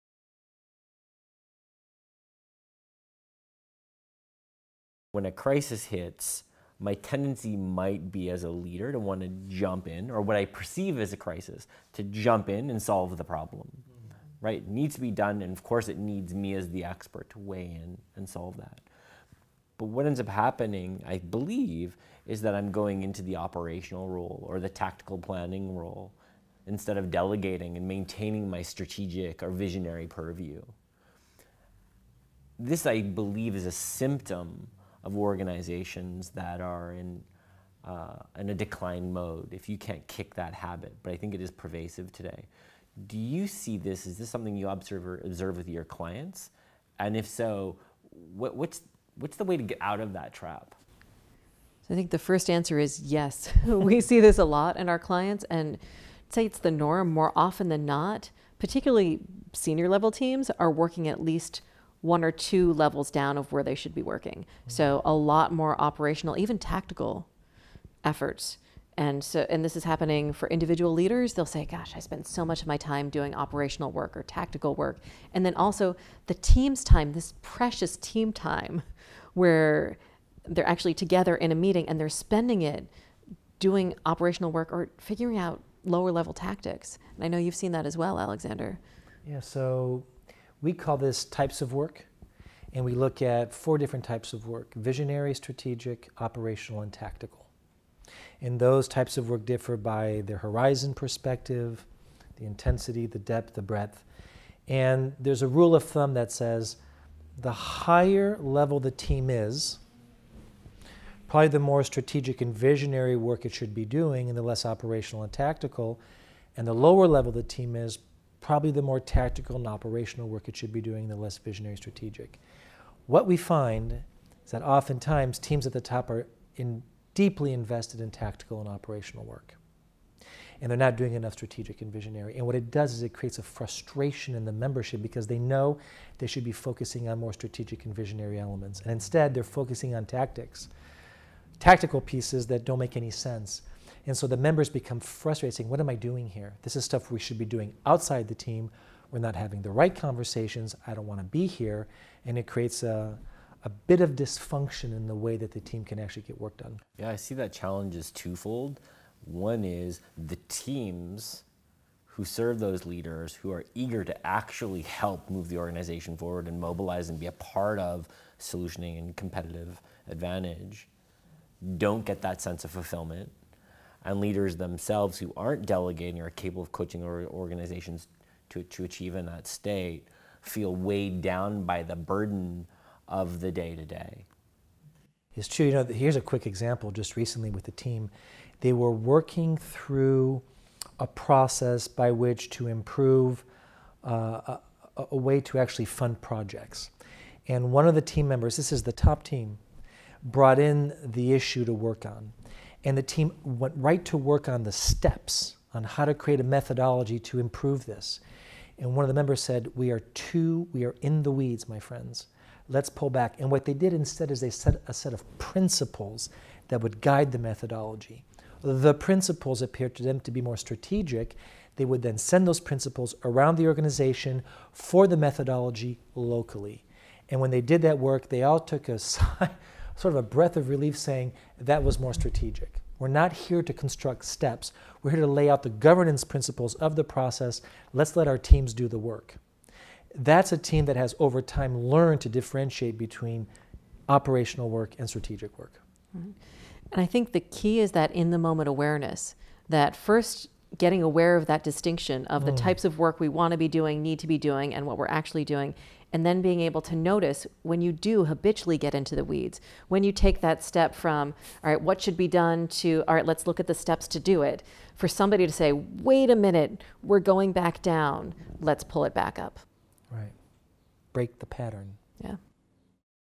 Helping Teams Stay Out of the Weeds | vivovii and Corentus
Many high-level teams devote far too little attention to strategic or visionary work, as their time and energy get consumed by operational and/or tactical work. Stephen Jones of vivovii, together with Alexander Caillet and Amy Yeager from Corentus, explores how teams can detect and address this challenge.

Come visit us at Corentus.com

Additional Free Resources:
Corentus.com/stateofmind
Corentus.com/accountability
Corentus.com/transforming-complaints
Corentus.com/resources